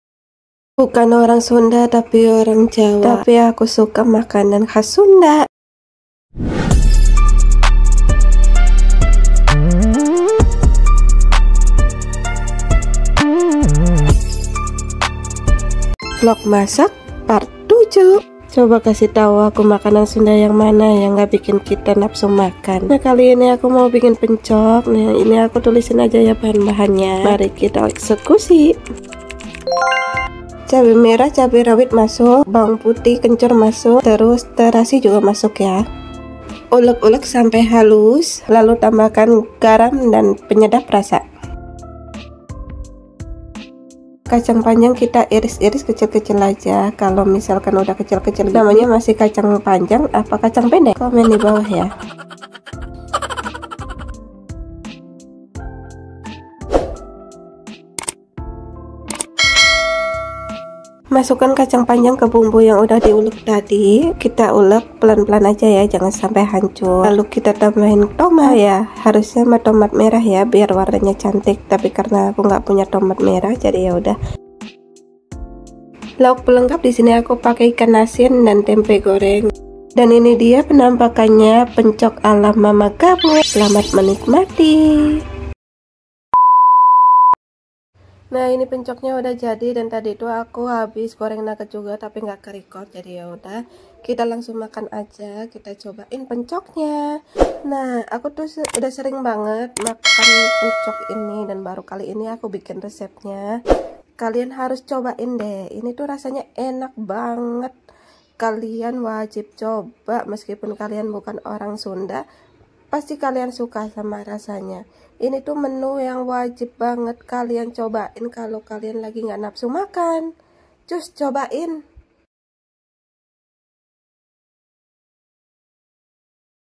BIKIN PENCOK KACANG PANJANG ❗❗ bukan orang sunda tapi suka
kali ini mamah gabut bikin pencok kacang panjang, siapa disini yg suka sama menu Sunda 1 ini?? ini resep dari teh @AndiniOverfield
nonton youtube nya teh Andini langsung pengen ehh sekalinya nyoba suka. izin tag ya teh hampura.